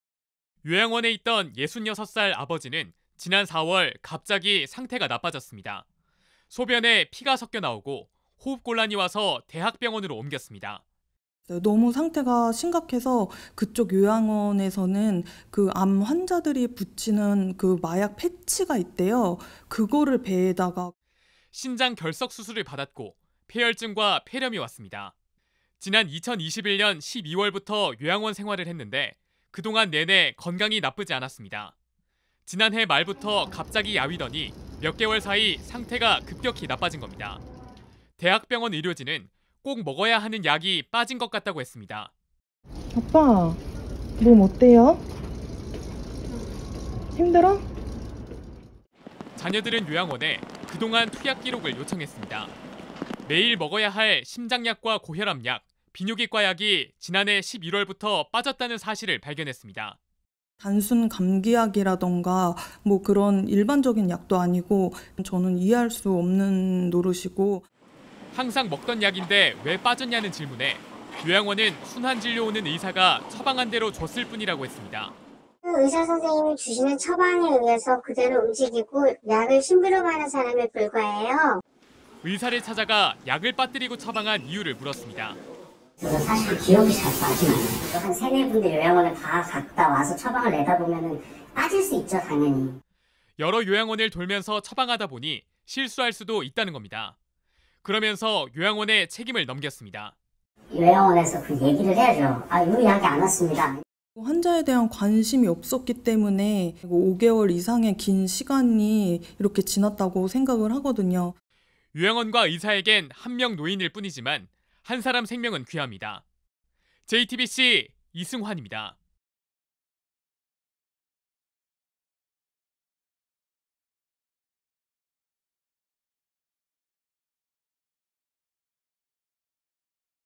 [자막뉴스] 대학병원서 들은 청천벽력 소견, 요양원에 물었더니 하는 말이 / JTBC News
요양원에서 지내던 60대 남성이 몇 달 사이 수술을 받아야 할 정도로 상태가 갑자기 나빠졌습니다. 수술을 한 대학병원에서 '꼭 먹어야 할 약을 안 먹은 것 같다' 해 알아보니, 정말로 5달 동안 못 먹은 걸로 드러났습니다. 자녀들의 항의에 요양원은 '순환 진료 오는 의사가 처방한 대로 줬다', 의사는 '여러 요양원을 돌다 보니 실수할 수도 있다'는 식이었는데, 이승환 기자가 취재했습니다.